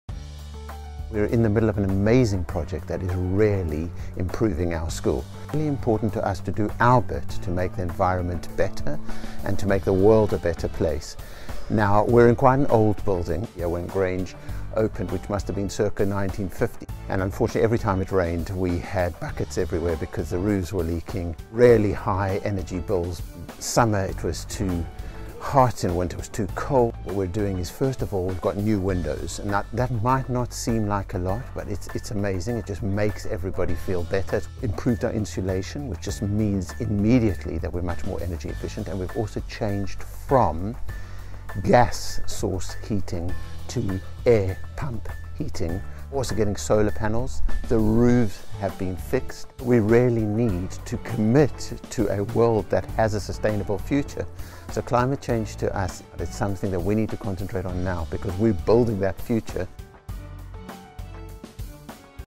Grange�20primary�20retrofit 2022 06 27 10 53 35
Script:

We’re in the middle of an amazing project that is really improving our school.
It’s really important to us to do our bit to make the environment better
And to make the world a better place.

Now, we’re in quite an old building here,
When Grange opened it must have been circa 1950
And unfortunately every time it rained
We had buckets everywhere because the roofs were leaking
Really high energy bills, summer it was too hot
And winter it was too cold.

What we’re doing, is first of all we’ve got new windows
Now that might not seem like a lot
But it’s amazing,
It just makes everybody feel better.
We’ve improved our insulation,
Which just means immediately that we’re much more energy efficient.
We’ve also changed from gas source heating to air pump (source) heating,
We’re also getting solar panels,
The roofs have been fixed.

We really need to commit to a world that has a sustainable future.
So climate change to us is something that we need to concentrate on now
Because we’re building that future.